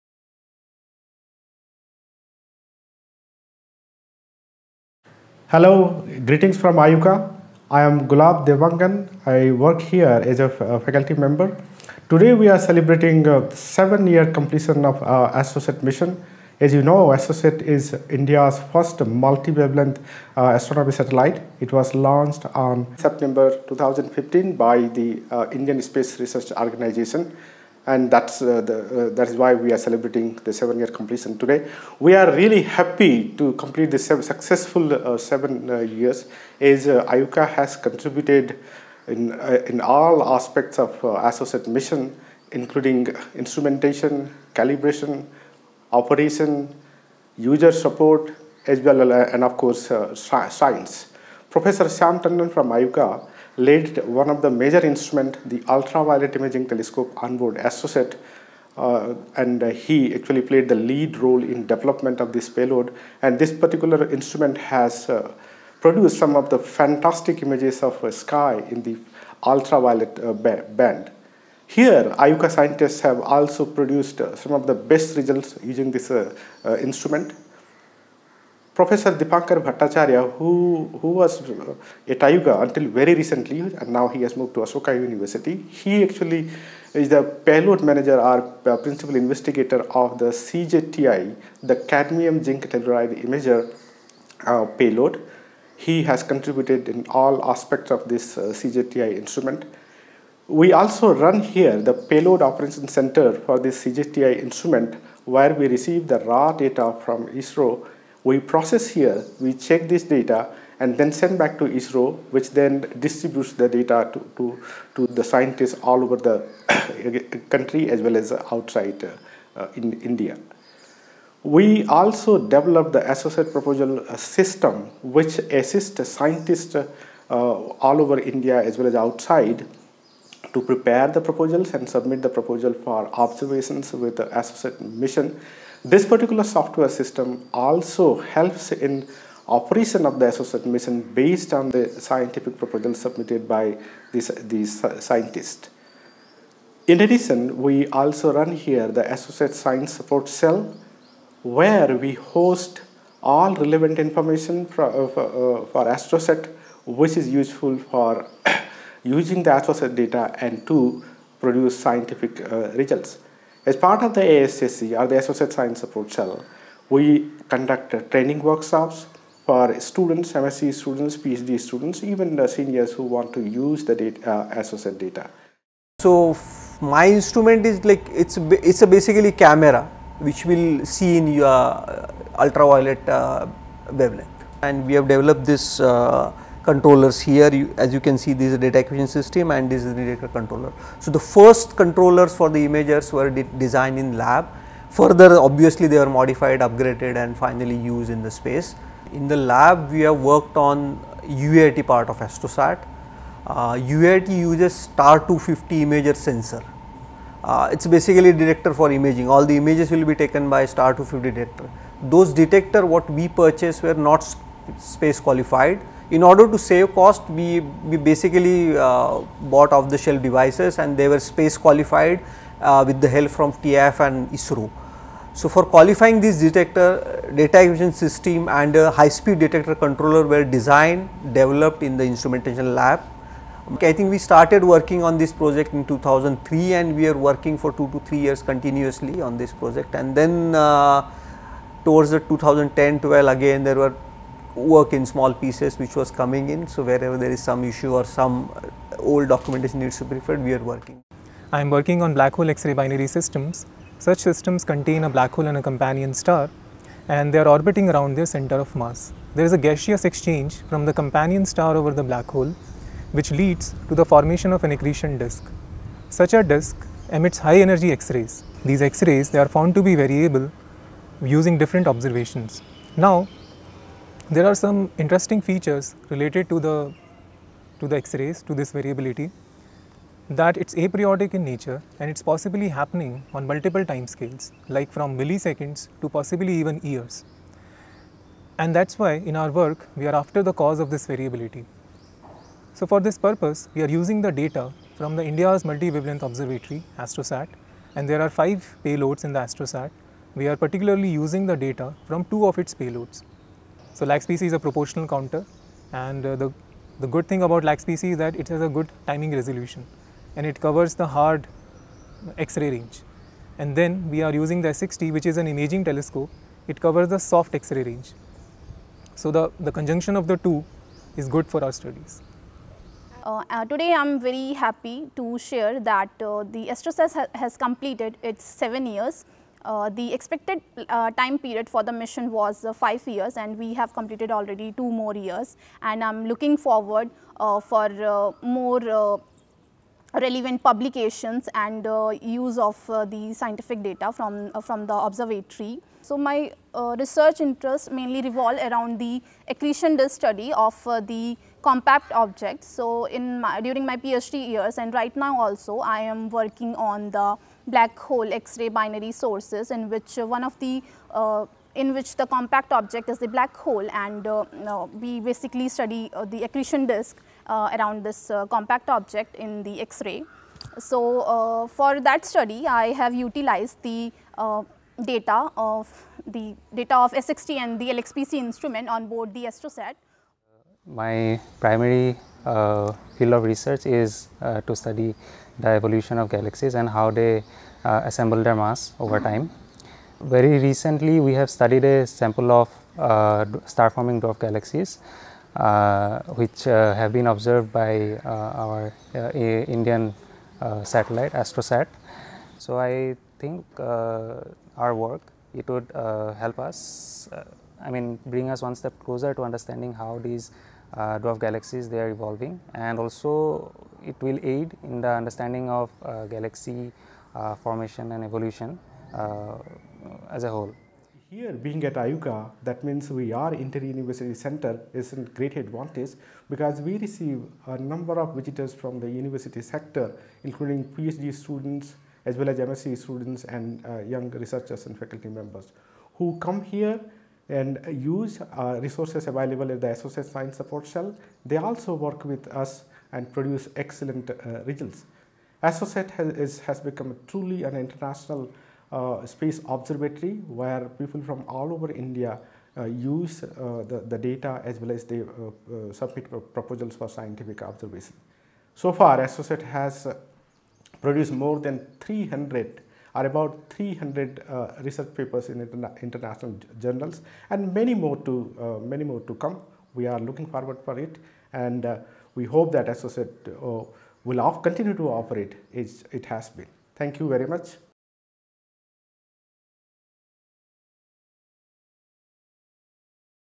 AstroSat @ 7 Years
IUCAA Celebrates 7 years of AstroSat's journey in Science and Astronomy. Our scientists talk about the role of IUCAA in AstroSat and also about the exciting discoveries made by it!